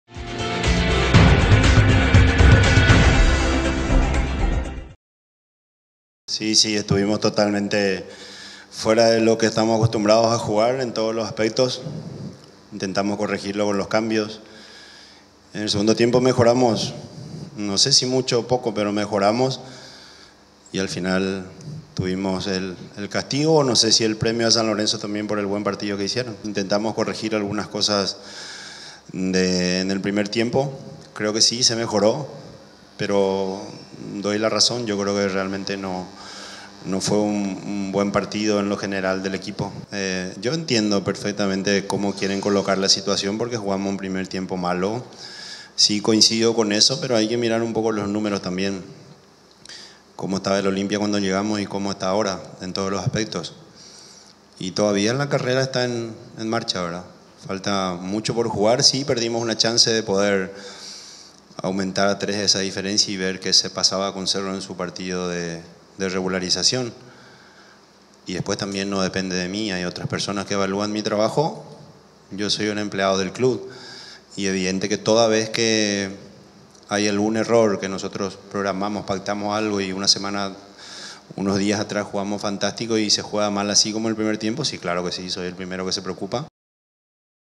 Sportivo San Lorenzo 1 vs. Olimpia 1 - Declaraciones del DT Francisco Arce
Declaraciones del entrenador Francisco Arce luego del empate de Olimpia ante el Sportivo San Lorenzo, 1-1, en el encuentro de este domingo de la novena fecha del torneo Clausura.